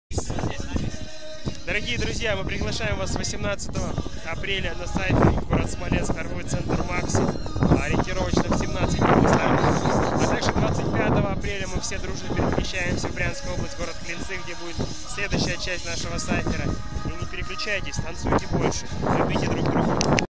приглашение из Брянска на"ORIGINAL BBOYS CYPHERS vol.23,24"
приглос на :_
18.04.15-"ORIGINAL BBOYS CYPHERS vol.23" MAXI 2 Смоленск
25.04.15-"ORIGINAL BBOYS CYPHERS vol.24" Клинцы Брянская область

простите всё фристайлом за 1 дубль...

Smolensk
BreaKing
Community
TELE
VISION